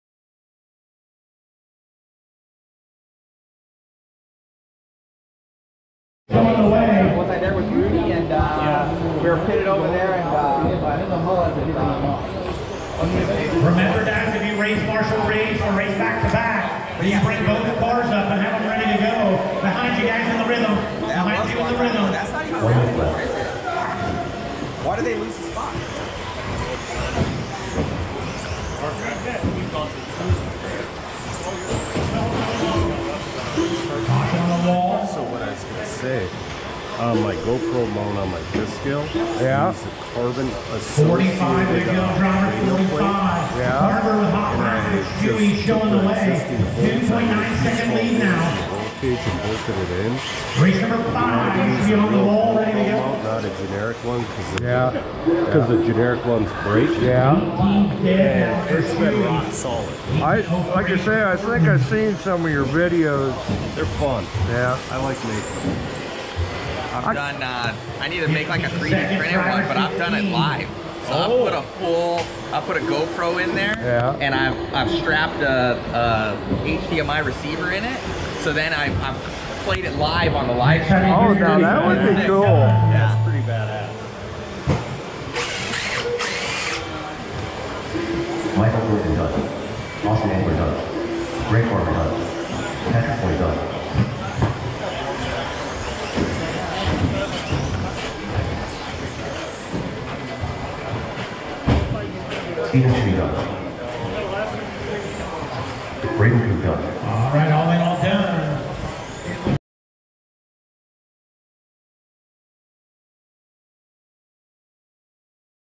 13.5 Stadium Truck qual2heat1 - NCTS5 Finals - Island Speedway Offroad, Muscatine, IA - 10-21-2023
Qualifiers and mains recorded from my hat cam of 1:10 scale carpet offroad racing during the JConcepts National Carpet and Turf Series 5  finals held at Star Collectibles/Island Speedway Offroad in Muscatine, Iowa on October 19th to the 22nd, 2023. The qualifiers I recorded took place on Saturday the 21st and the mains on Sunday the 22nd.